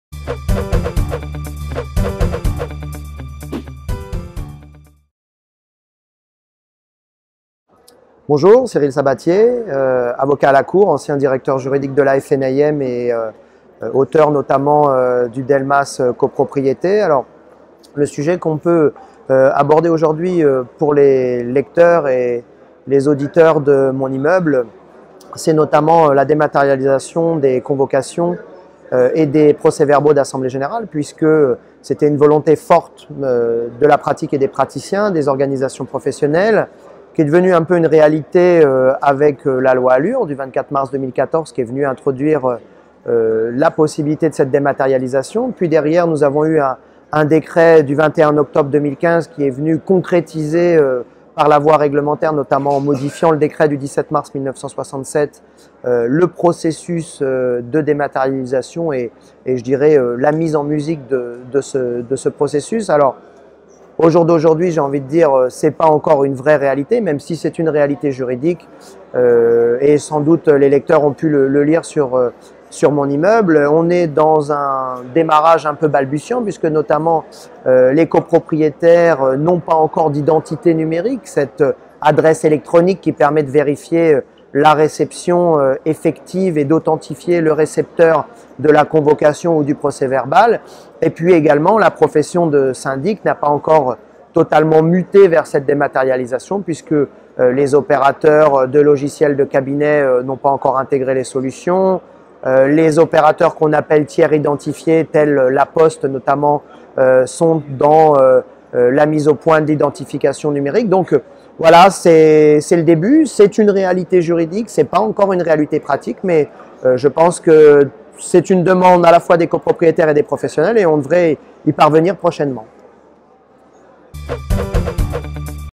Cyril SABATIE : La dématérialisation des notifications en copropriété
Interview réalisé par Mon-immeuble.com à l'occasion du congrès de l’immobilier 2015. Cyril Sabatié, Avocat à la cour et expert Immobilier, associé au sein du Cabinet LBVS AVOCATS.